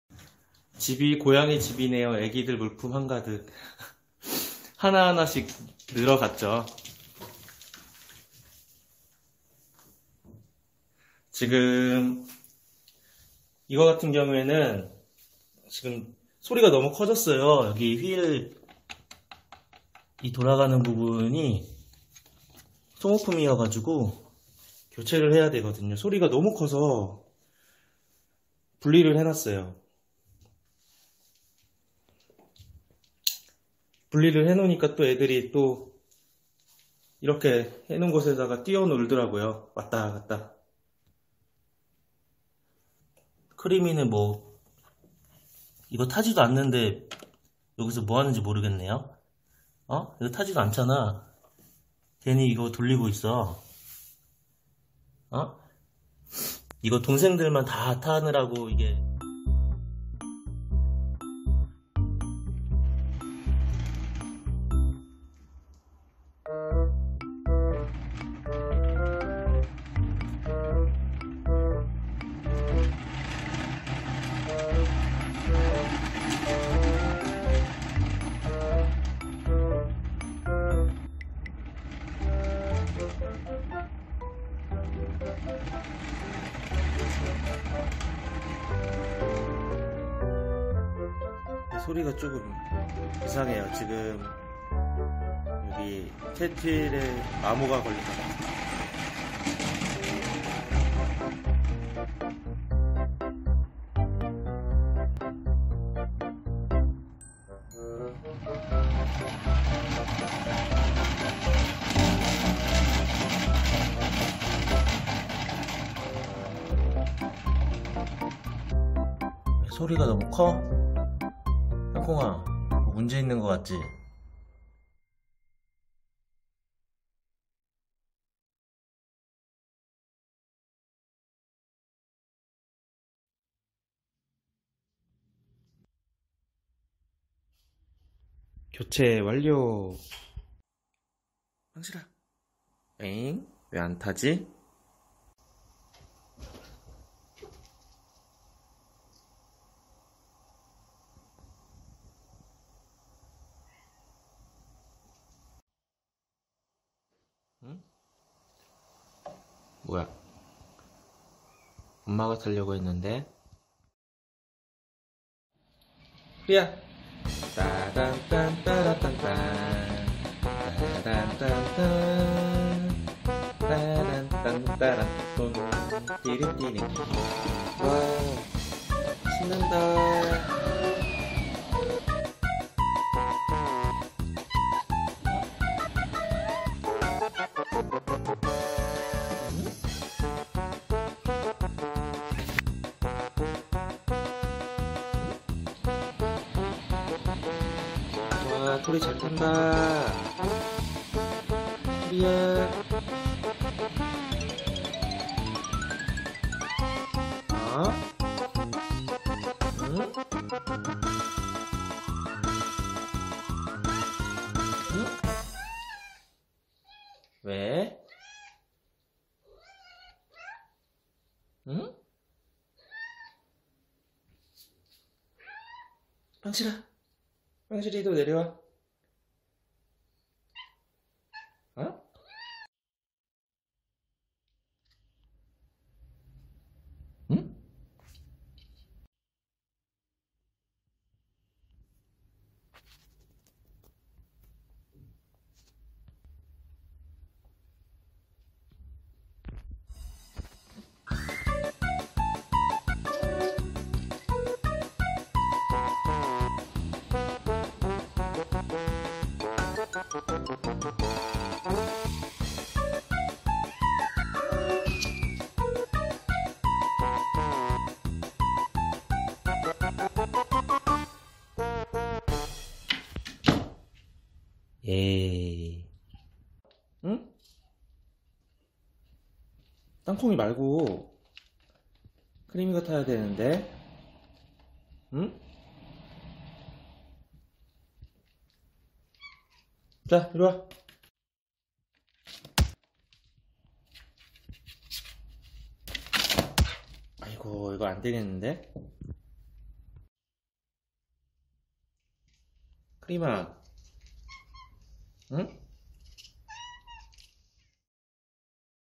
순딩한 빵실이네도 시끄러워요
고양이 캣휠 쏠림 베어링이 마모 되어서 소음이 심하게 났어요..새벽마다 캣휠 소리에 잠에서 깨기도 하고^^;; 땅콩 토리의 캣휠 사랑은 어쩔 수 없네요! 빵실이와 크림이와 함께 사냥놀이도 함께해요